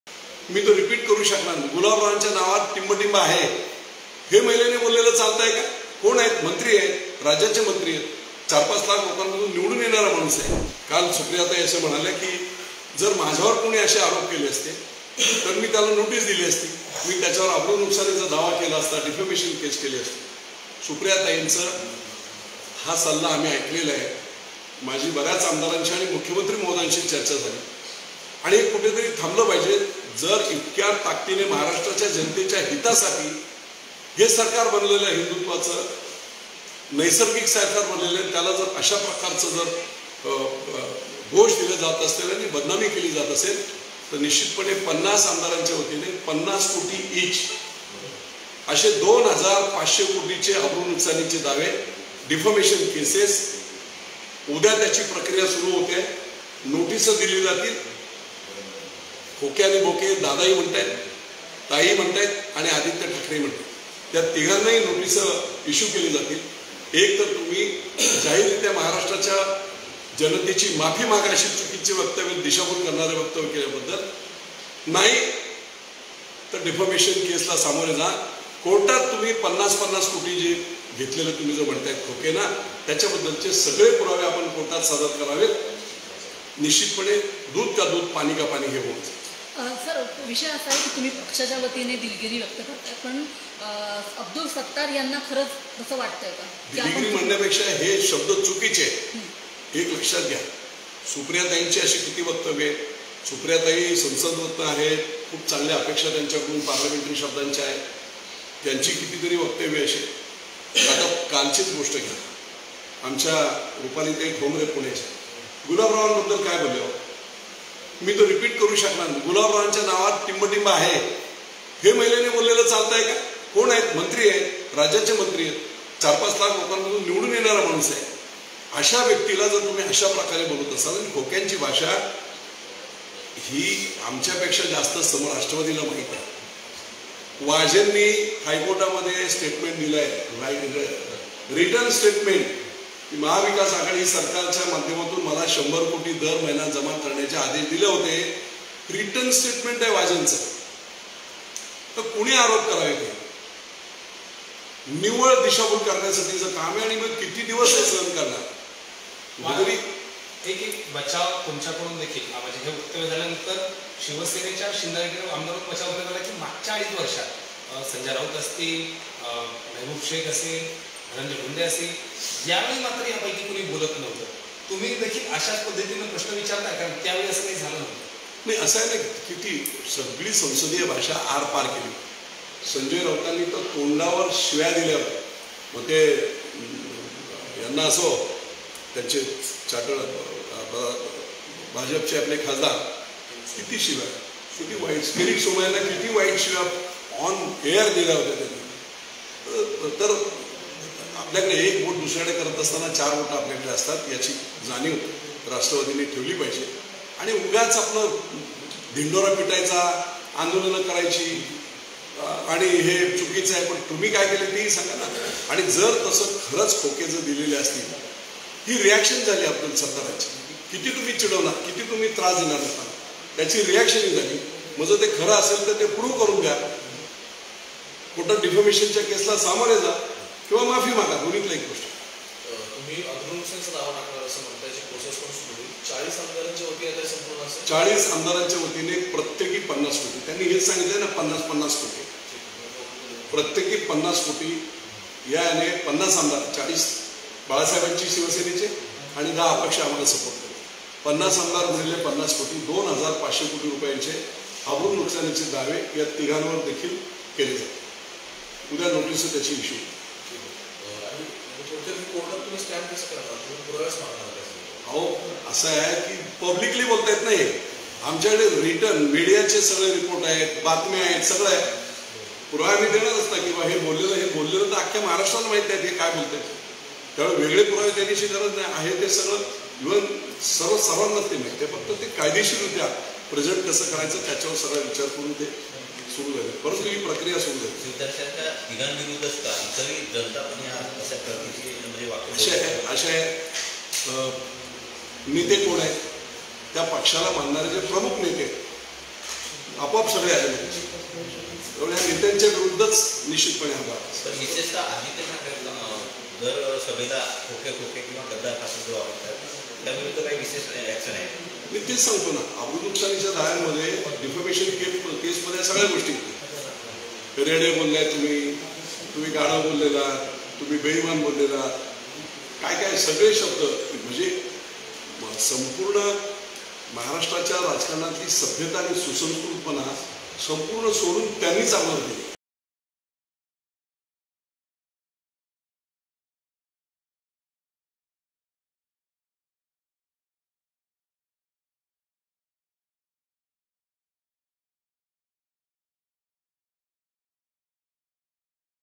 Vijay Shivtare : 'आरोप करणाऱ्यांविरुद्ध अब्रुनुकसानीच्या केसेस दाखल करणार' | LetsUpp Marathi
मुख्यमंत्री एकनाथ शिंदे गटाकडून आता आमदारांवर ५० खोक्यांबाबत आरोप करणाऱ्यांविरुध्द अब्रु नुकसानीच्या केसेस दाखल करण्याची तयारी सुरू करण्यात आलीय.  याबाबत शिंदे गटाच्या प्रवक्ते विजय शिवतारे यांनी ही माहिती दिली